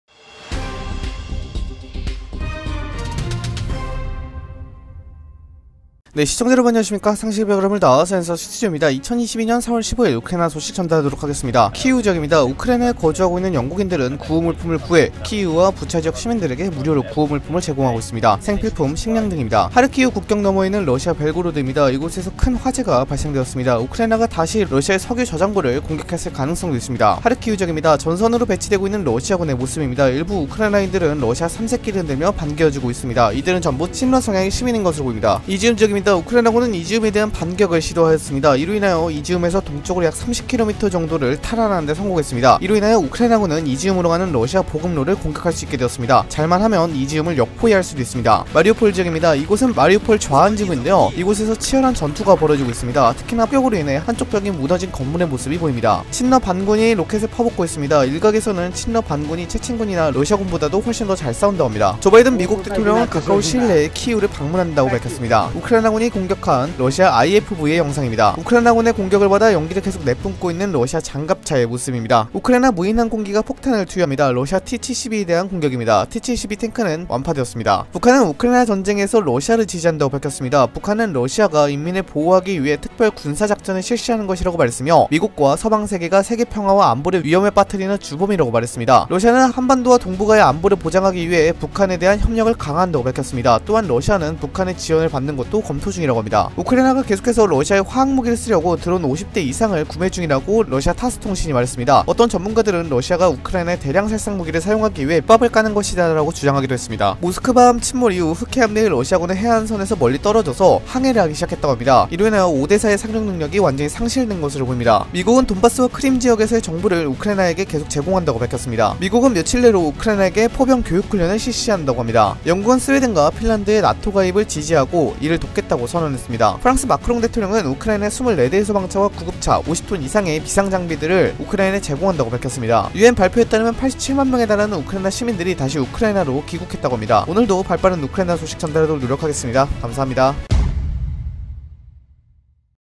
[속보] 북한, 러시아의 침공 행위는 정당... 러시아 지원 한다 _우크라이나 긴급속보169_[센서 스튜디오]
키이우 지역입니다. 우크라이나에 거주하고 있는 영국인들은 구호물품을 구해 키이우와 부차 지역 시민들에게 무료로 구호물품을 제공하고 있습니다. 생필품, 식량 등입니다 - 더 썬

하르키우 국경 너머에 있는 러시아 벨고로드입니다. 이곳에서 큰 화재가 발생되었습니다. 우크라이나가 다시 러시아의 석유저장고를 공격했을 가능성도 있습니다. - 우크라이나 현지 제보(텔레그렘)

하르키우 지역입니다. 전선으로 배치되고 있는 러시아군의 모습입니다. 일부 우크라이나인들은 러시아 삼색기를 흔들며 반겨주고 있습니다. 이들은 전부 친러성향의 시민인 것으로 보입니다.  - 우크라이나 현지 제보(텔레그렘)

이지움 지역입니다. 우크라이나군은 이지움에 대한 반격을 시도하였습니다. 이로 인하여 이지움에서 동쪽으로 약 30km 정도를 탈환하는데 성공했습니다. 이로 인하여 우크라이나군은 이지움으로 가는 러시아 보급로를 공격할 수 있게 되었습니다. 잘만하면 이지움을 역포위할 수도 있습니다 - 마이클 맥케이

마리우폴 지역입니다. 이곳은 마리우폴 좌안 지구인데요. 이곳에서 치열한 전투가 벌어지고 있습니다. 특히나 포격으로 인해 한쪽 벽이 무너진 건물의 모습이 보입니다.  - 우크라이나 현지 제보(텔레그렘)

친러반군이 로켓을 퍼붓고 있는 모습입니다. 일각에서는 친러반군이 체첸군이나 러시아군보다도 훨씬 더 잘싸운다고 합니다. - 우크라이나 현지 제보(텔레그렘)

조 바이든 미국 대통령은 가까운 시일내에 키이우를 방문한다고 밝혔습니다 - NEXTA

우크라이나군이 공격한 러시아 IFV의 영상입니다 - 우크라이나 현지 제보(텔레그렘)

우크라이나군의 공격을 받아 연기를 계속 내뿜고 있는 러시아 장갑차의 모습입니다. - 우크라이나 현지 제보(텔레그렘)

우크라이나의 무인항공기가 폭탄을 투하합니다. 러시아 T-72에 대한 공격입니다. T-72 탱크는 완파되었습니다.  - 우크라이나 현지 제보(텔레그렘)

북한은 우크라이나 전쟁에서 러시아를 지지한다고 밝혔습니다. 북한은 러시아가 인민을 보호하기 위해 특별군사작전을 실시하는 것이라고 말했으며, 미국과 서방세계가 세계 평화와 안보를 위험에 빠뜨리는 주범이라고 말했습니다. 러시아는 한반도와 동북아의 안보를 보장하기 위해 북한에 대한 협력을 강화한다고 밝혔습니다 또한 러시아는 북한의 지원을 받는 것을 검토중이라고 합니다.- 타스 통신

우크라이나가 계속해서 러시아에 화학무기를 쓰려고 드론 50대 이상을 구매중이라고 러시아 타스통신이 말했습니다. 어떤 전문가들은 러시아가 우크라이나에 대량살상무기를 사용하기 위해 밑밥을 까는 것이다라고 주장하기도 했습니다. - 타스통신

모스크바함 침몰 이후 흑해함대의 러시아군은 해안선에서 멀리 떨어져서 항해를 하기 시작했다고 합니다. 이로 인하여 오데사의 상륙 능력이 완전히 상실된 것으로 보입니다. - 이드리스 알리

미국은 돈바스와 크림 지역에서의 정보를 우크라이나에게 계속 제공한다고 밝혔습니다 - CNN

미국은 며칠내로 우크라이나군에게 포병 교육 훈련을 실시한다고 합니다 - 잭 데치

영국은 스웨덴과 핀란드의 NATO가입을 지지하고 이를 돕겠다고 선언했습니다 - CNN

프랑스 마크롱 대통령은 우크라이나에 24대의 소방차와 구급차, 50톤 이상의 비상장비들을 우크라이나에 제공한다고 밝혔습니다. - 에마누엘 마크롱

UN 발표에 따르면 87만명에 달하는 우크라이나 시민들이 다시 우크라이나로 귀국했다고 합니다 - NEXTA